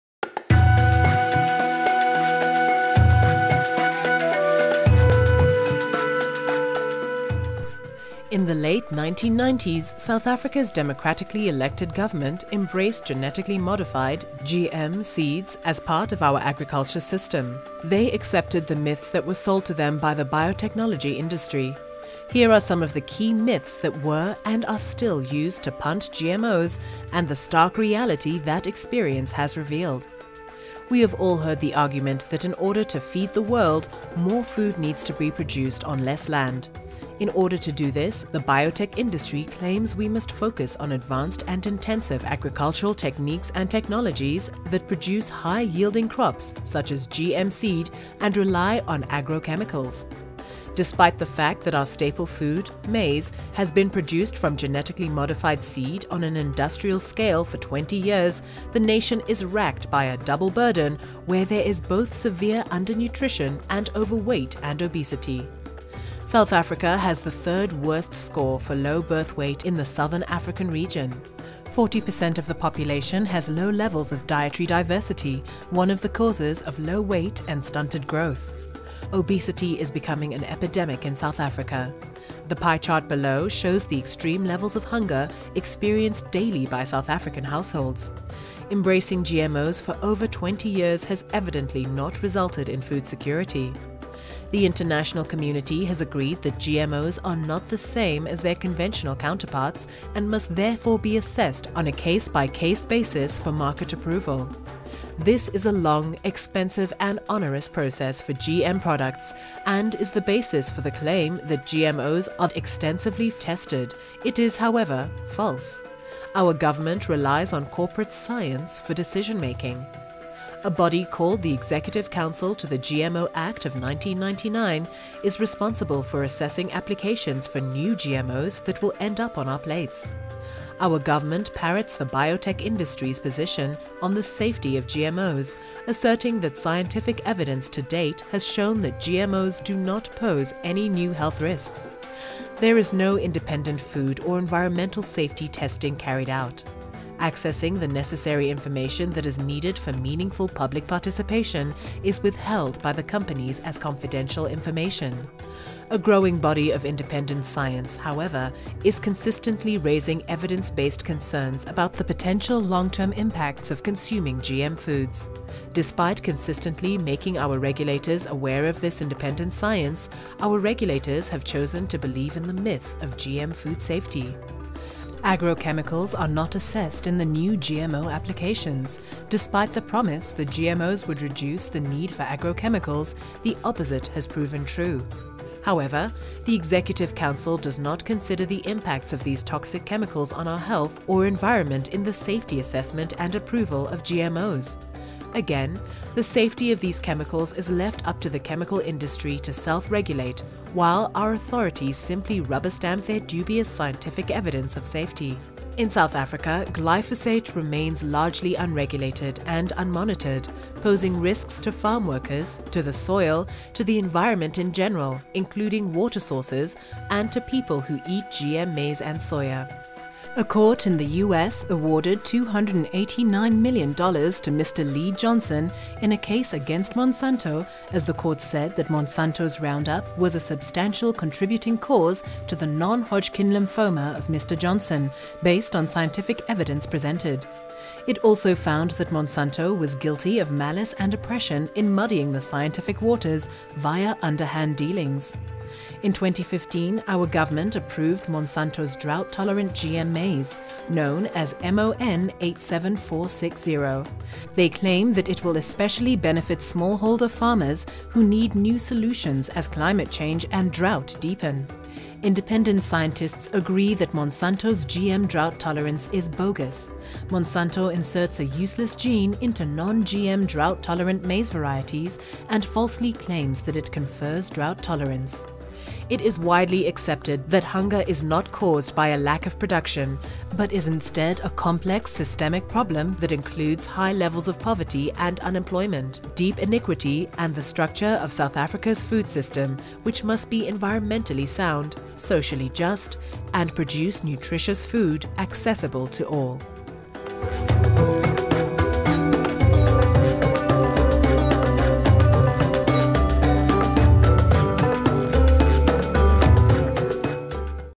Debunking Myths Relating to GMOs 3
The African Centre for Biodiversity (ACB) is pleased to share with you a three-part animation series on GMOs in South Africa.
The animations speak to the following topics:
1. History of Maize Cultivation and Consumption in South Africa
2. GMOs in South Africa
3. Debunking Myths Relating to GMOs

All the animation voiceovers have been translated into and are available in the following languages (please scroll though the list to get to your language preference):
isiXhosa
isiZulu
SeSotho
Afrikaans

Please do share these animations with your networks.